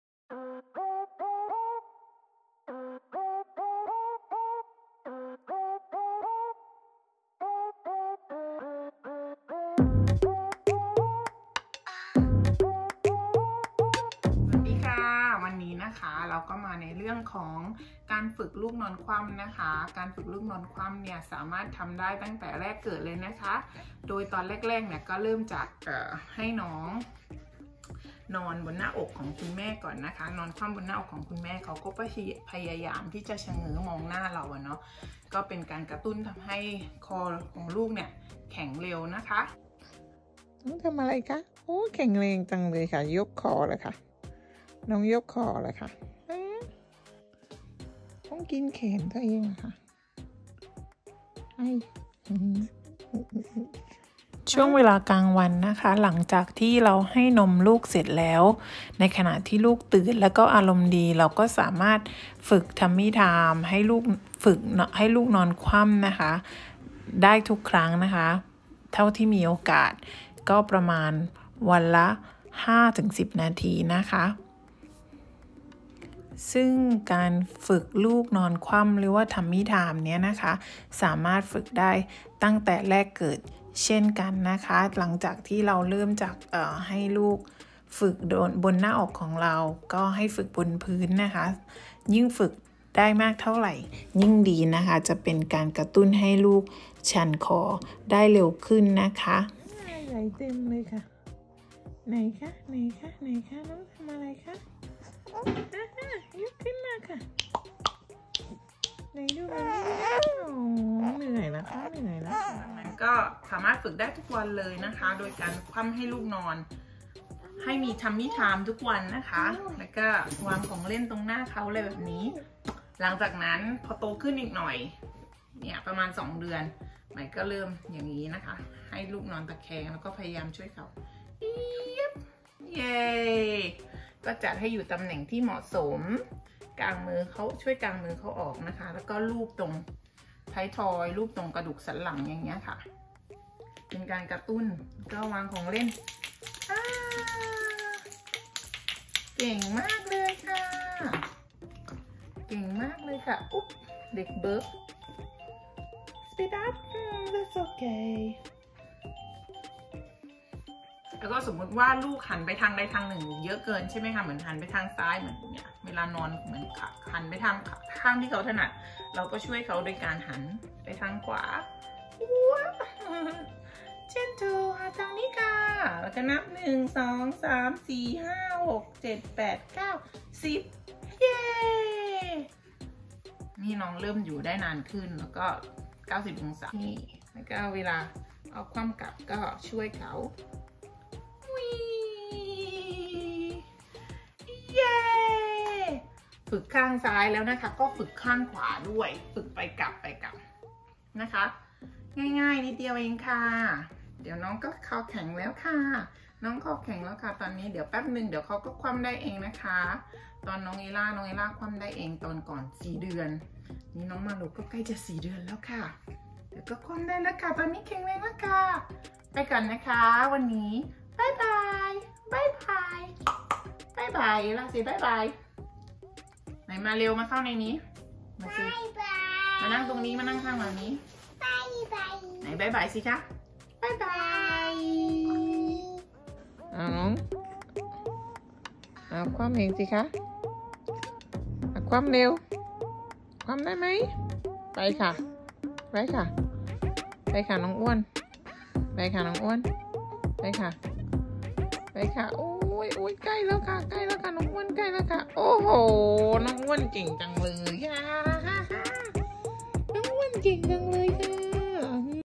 การฝึกลูกนอนคว่ำ | Tummy time
ทุกครั้งที่ฝึกต้องฝึกขณะที่ลูกอารมณ์ดี เป็นกำลังใจให้คุณแม่ทุกคนค่ะ